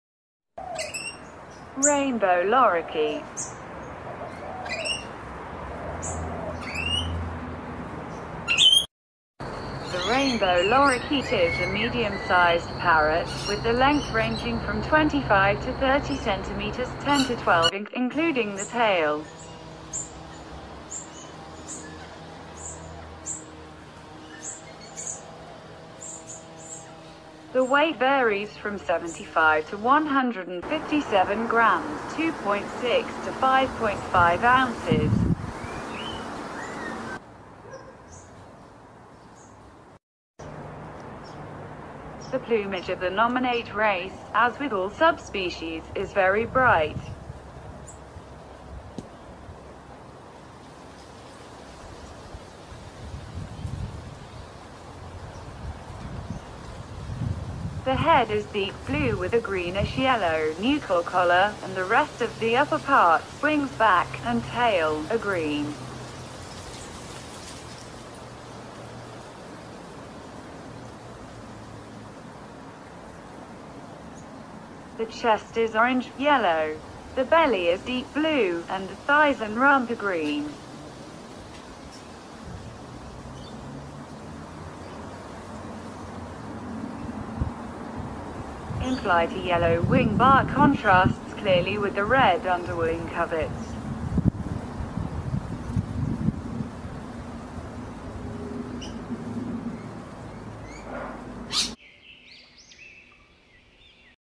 Rainbow lorikeet. Trichoglossus moluccanus
Rainbow Lorikeets
The rainbow lorikeet is a medium-sized parrot, with the length ranging from 25 to 30 cm (10 to 12 in), including the tail. The weight varies from 75 to 157g (2.6–5.5 oz). The plumage of the nominate race, as with all subspecies, is very bright. The head is deep blue with a greenish-yellow nuchal collar, and the rest of the upper parts (wings, back, and tail) are green. The chest is orange/yellow. The belly is deep blue, and the thighs and rump are green. In-flight a yellow wing-bar contrasts clearly with the red underwing coverts.